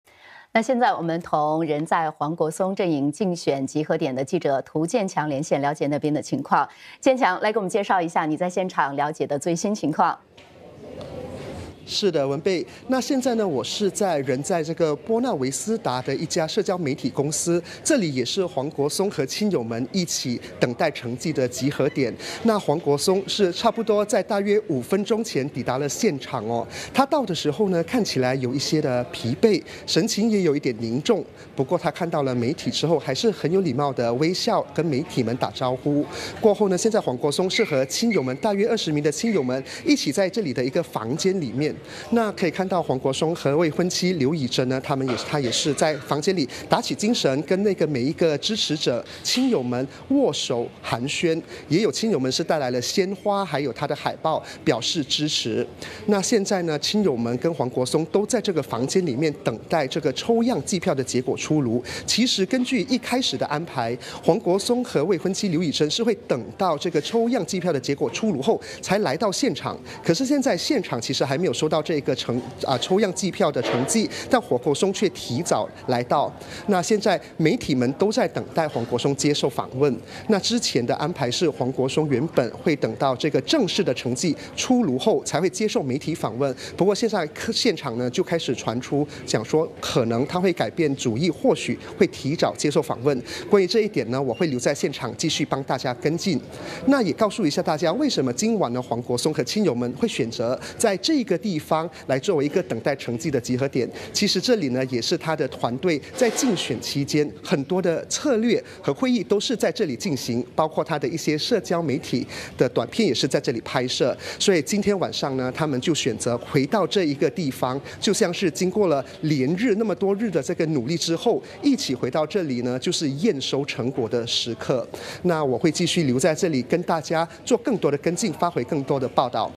【新加坡总统选举2023】黄国松与未婚妻同每位到场的支持者握手寒暄
黄国松与未婚妻同每位到场的支持者握手寒暄